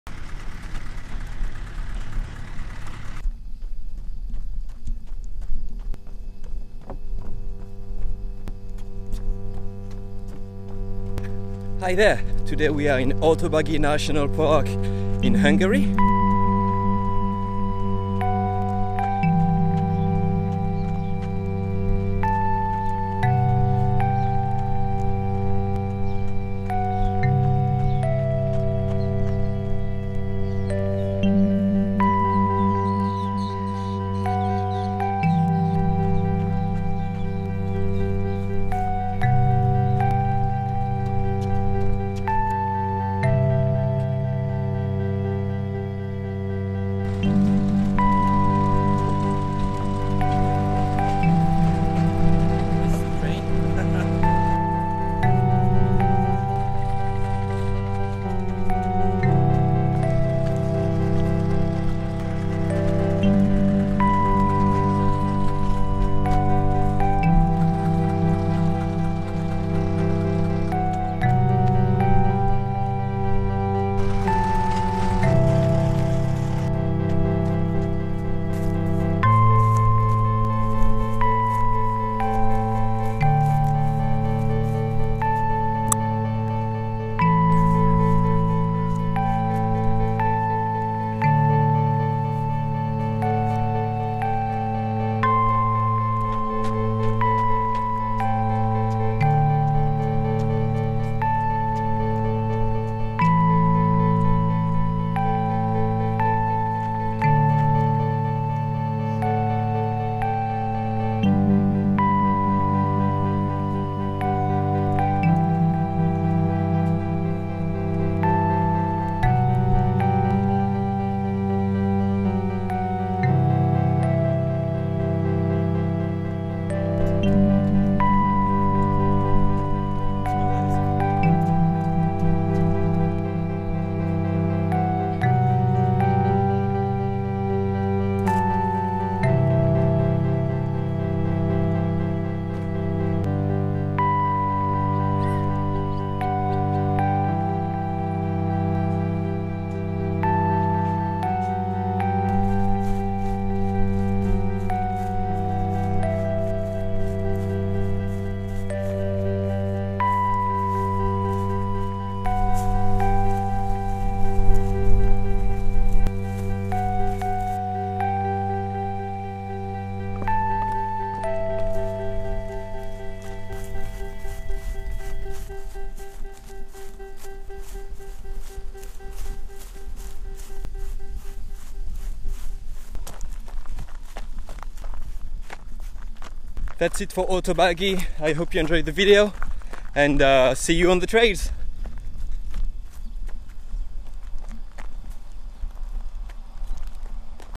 Hortobagyi in Hungary - Runnexplorer 36
Hortobagyi is an Hungarian National Park famous for its bird reserve.
I play "catch" with the touristic train for a while before heading to the less traveled paths of the park.

Music

Music used under a Creative Common 3.0 license or used with written Author agreement.